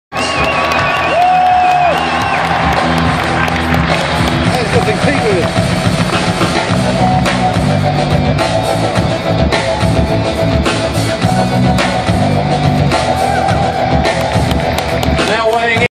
Davey Grant vs. Roland Delorme UFC London weigh-in
Davey Grant and Roland Delorme square off ahead of their bantamweight clash at UFC Fight Night in London